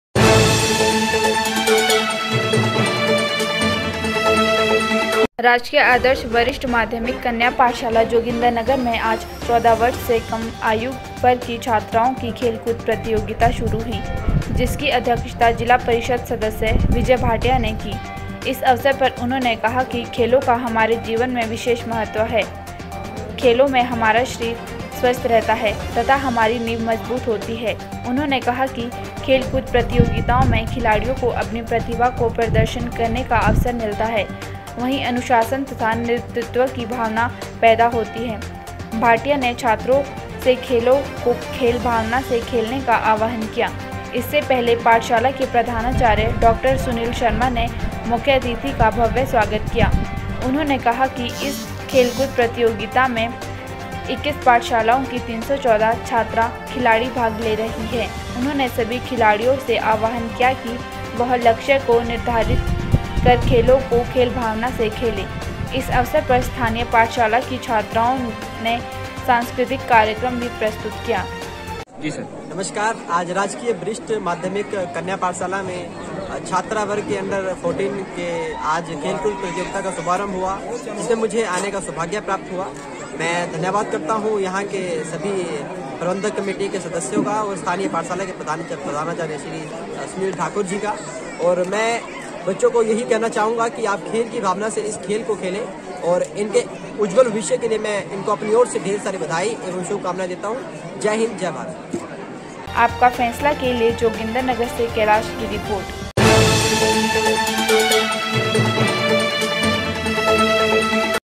जोगिंदरनगर में 14 वर्ष से कम आयु वर्ग की छात्राओं की खेलकूद प्रतियोगिता शुरू....
राजकीय आदर्श वरिष्ठ माध्यमिक  कन्या पाठशाला जोगिंदरनगर में आज 14वर्ष से कम आयु वर्ग की छात्राओं की खेल कूद प्रतियोगिता शुरू हुई,जिसकी अध्यक्षता जिला परिषद सदस्य विजय भाटिया ने की।इस अवसर पर उन्होंने कहा कि खेलों का हमारे जीवन में विशेष महत्व है।खेलों से हमारा शरीर स्वस्थ रहता है तथा हमारी नींव मजबूत होती है। उन्होंने कहा कि खेलकूद प्रतियोगिताओं में खिलाड़ियों को अपनी प्रतिभा को प्रदर्शन करने का अवसर मिलता है, वहीं अनुशासन तथा नेतृत्व की भावना पैदा होती है। भाटिया ने  छात्राओं से  खेलों को खेल भावना से खेलने का आह्वान किया। इससे पहले पाठशाला के प्रधानाचार्य डॉक्टर सुनील शर्मा ने मुख्यातिथि का भव्य स्वागत किया।उन्होंने कहा कि इस खेलकूद प्रतियोगिता में 21पाठशालाओं की 314 छात्रा खिलाड़ी भाग ले रही है। उन्होंने सभी खिलाड़ियों से आह्वान किया कि वह लक्ष्य को निर्धारित कर खेलों को खेल भावना से खेलें। इस अवसर पर स्थानीय पाठशाला की छात्राओं ने सांस्कृतिक कार्यक्रम भी प्रस्तुत किया।